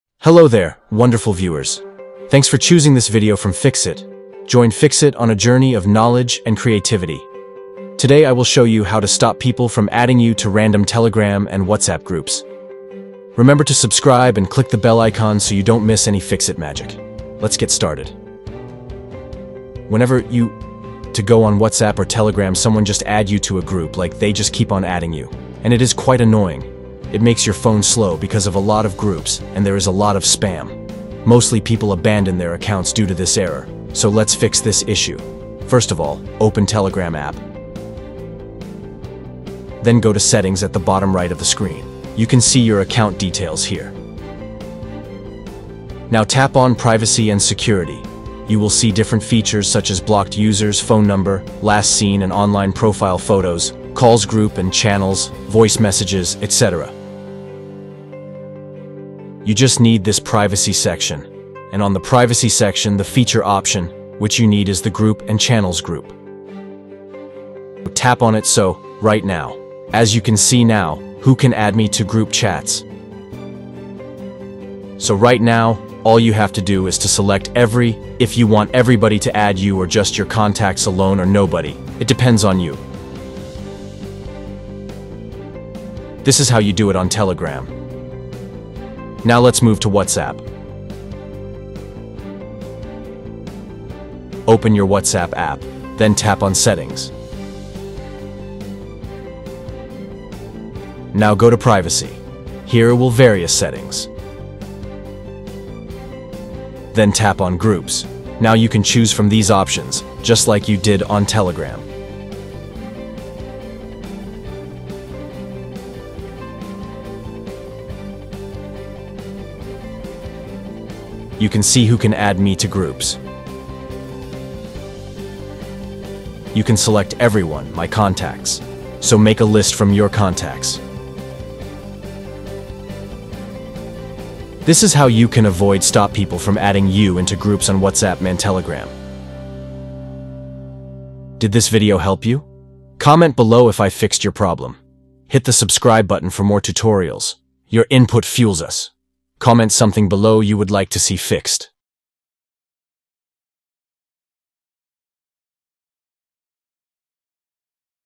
How To Stop People From Adding You In Random Telegram Groups
Have you ever got added to random groups unknowingly?

If you’re on Telegram, you must have faced this issue quite a lot of times. Sometimes other people just add you to random Telegram groups without our consent.

Most of the groups we are added into are spams.

It is quite frustrating to remain in that group when we don’t have any interest to get added in these groups.

No one likes getting added to groups, they are not interested in.

So how can you stop people from adding you in random Telegram groups?

It is all about changing the settings of your Telegram account.

The main thing is that Telegram works with both phone numbers and username, so it is an advantage to the spammers to add you in their groups.

We just don’t want to delete these groups again and again.

So, what can we do?

Just watch this video till the end and learn how you can stop people from adding you in any random Telegram group.